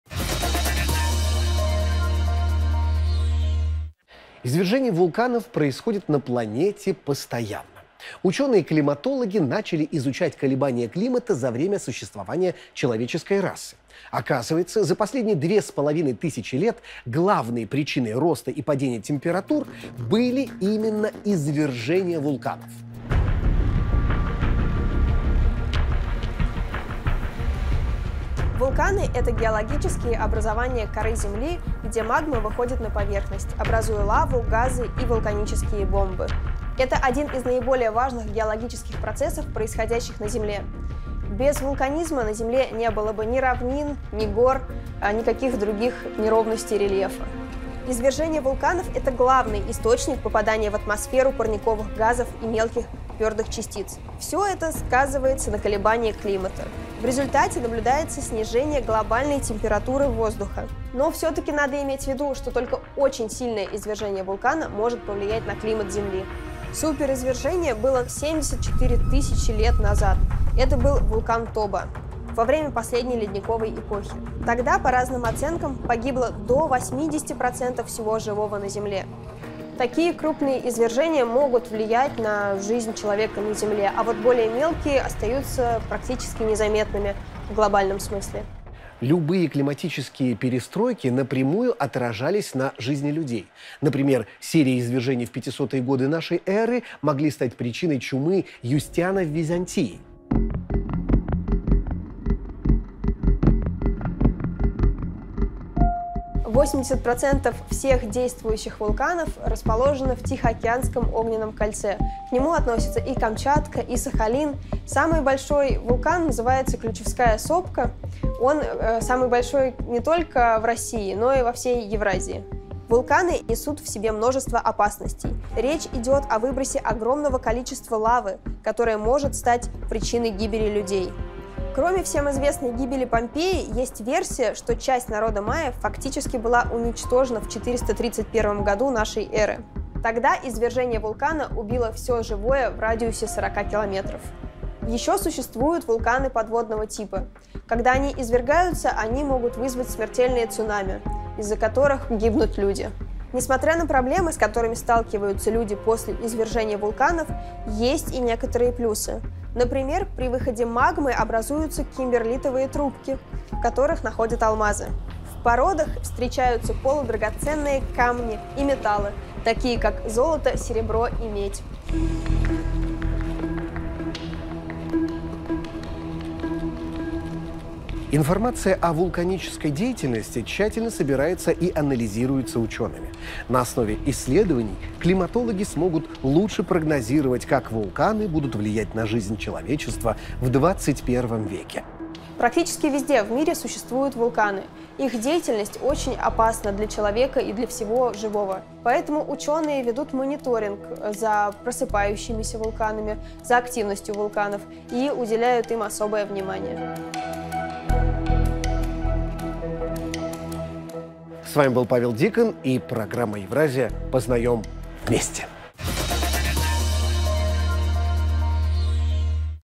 Тайны извержения вулканов. Какие глобальные перемены они несут человечеству?
Извержения вулканов происходят на планете постоянно. Ученые-климатологи начали исследовать колебания климата за время существования человеческой расы. Оказывается, за последние 2500 тысячи лет главной причиной роста и падения температур были именно извержения вулканов.